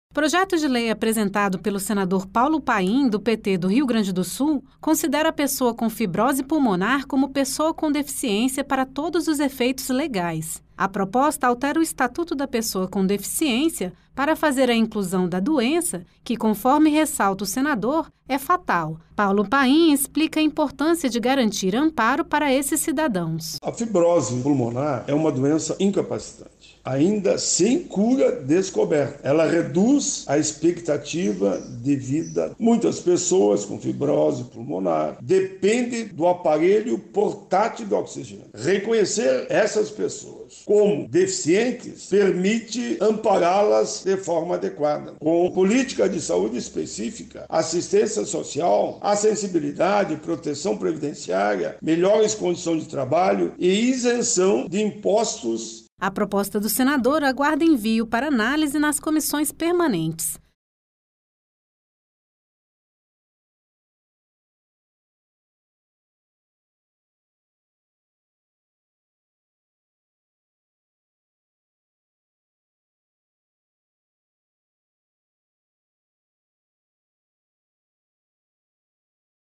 Proposta inclui fibrose pulmonar no Estatuto da Pessoa com Deficiência
Projeto de lei apresentado pelo senador Paulo Paim (PT-RS) prevê que as pessoas com fibrose pulmonar sejam consideradas, perante a legislação, como pessoas com deficiência. O PL 2.661/2022 altera o Estatuto da Pessoa com Deficiência (Lei 13.146, de 2015) de modo a garantir aos cidadãos diagnosticados com a doença todos os direitos previstos na lei.